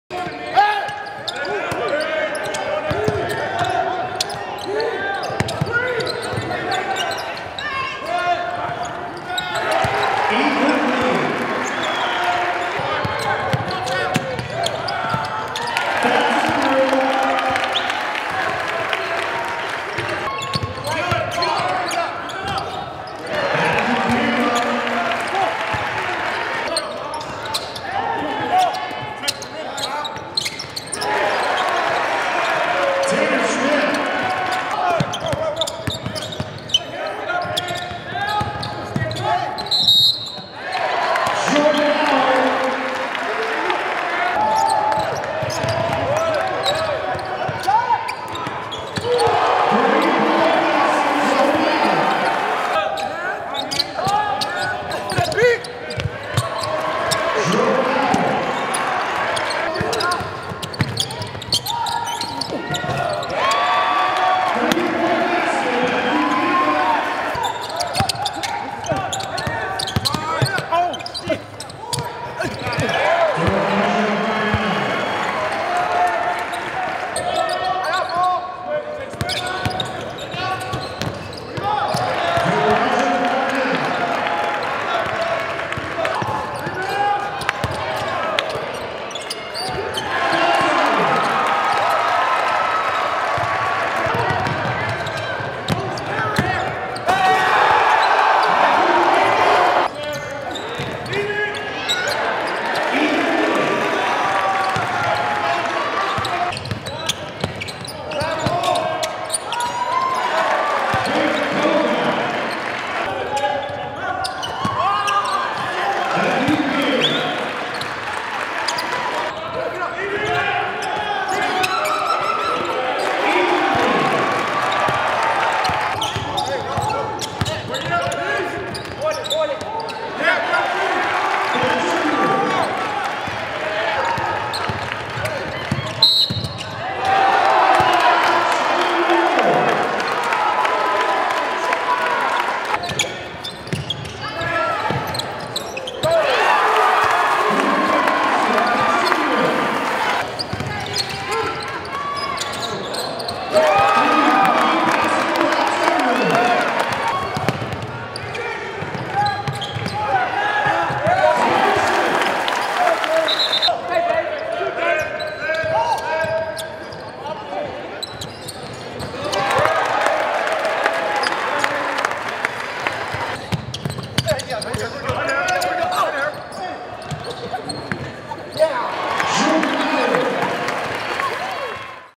Men's Basketball: Hiland Dairy Hilights, Texas A&M-Corpus Christi
The Hiland Dairy Hilights from UCA's 81-69 win over Texas A&M-Corpus Christi on Friday. Thatch Unruh led the Bears with 20 points. Jordan Howard added 18 points, 5 rebounds, 5 steals and 5 assists while Mathieu Kamba scored 13 points and grabbed 6 rebounds.

Next up for the Bears is Southeastern Louisiana at 3 p.m. Sunday in the Farris Center.